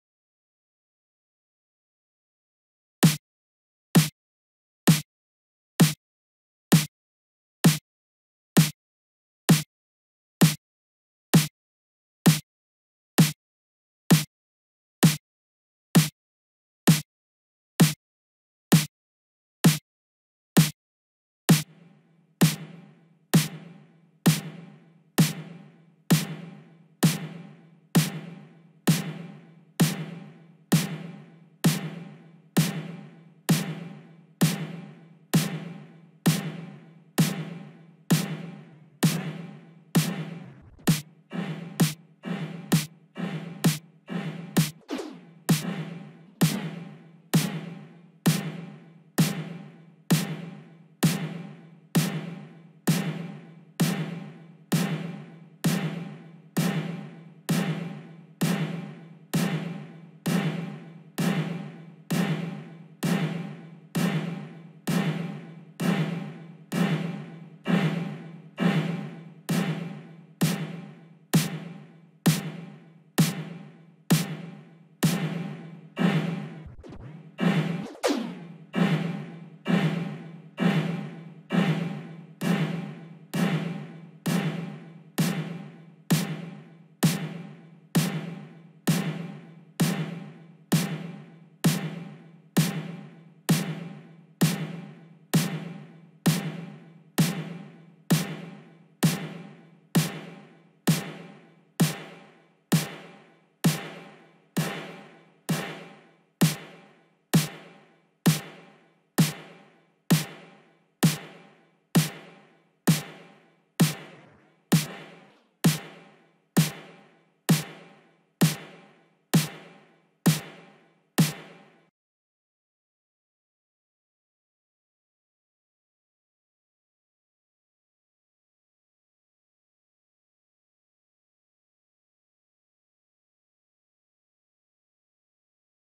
FREE RO-GOLD by Black Rooster Audio
RO-GOLD :
This physically modeled plug-in captures the true tone of a solid gold plate reverb, turning any signal into 24k magic. The most luxurious and exclusive vintage room effect meets modern-day features for the highest-quality sound and optimal usability.

Snare Sample by OSvaldo R.